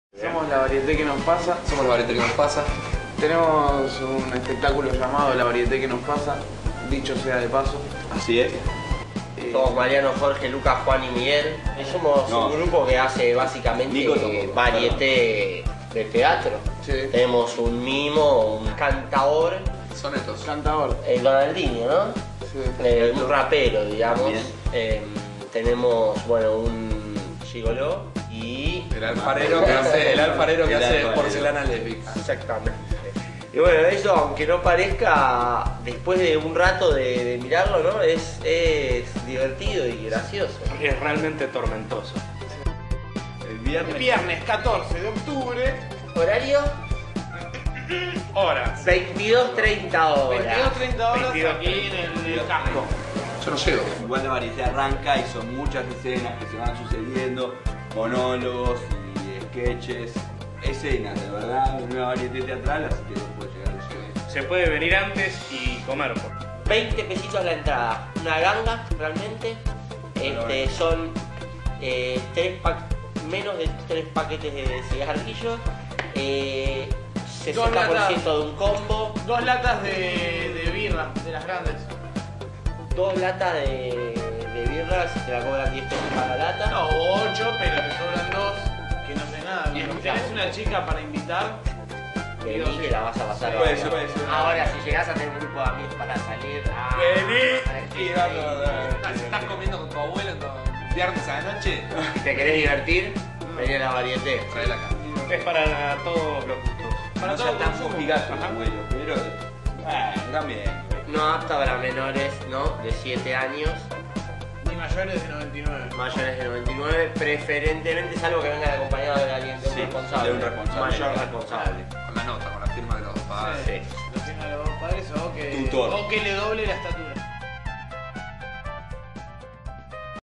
variete Que nos Pasa
nos comentan de que se trata, La variete, que nos pasa, Los viernes en el Centro Cultural del Casco, ex Arbol,